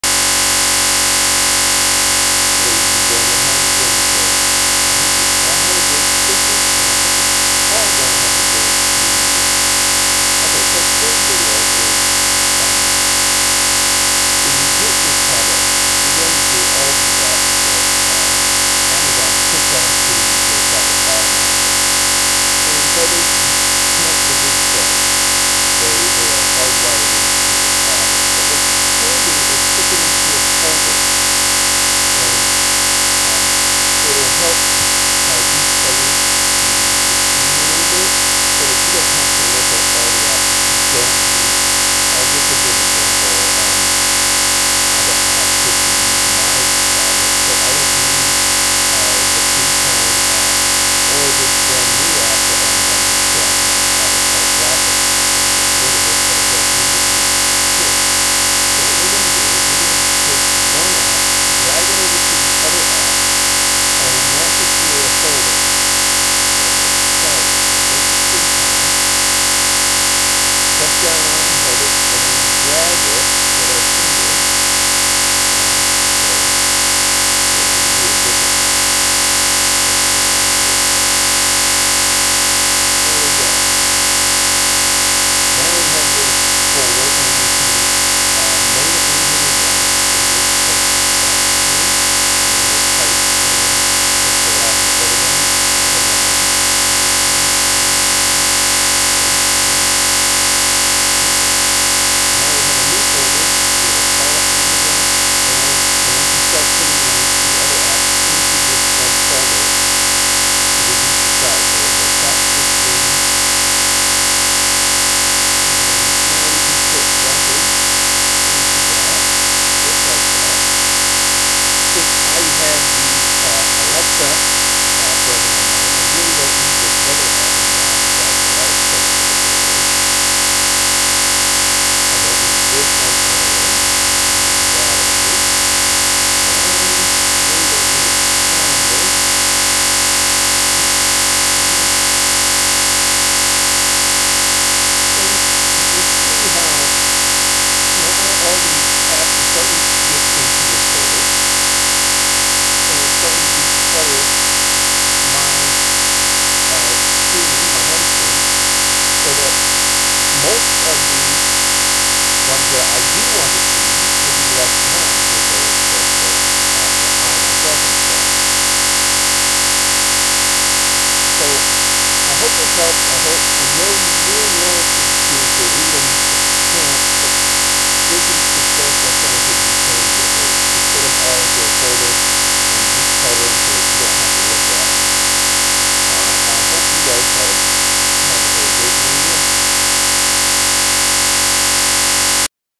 Decluttering the home screen of your kindle fire 7
Showing you how to move unwanted apps into a small folder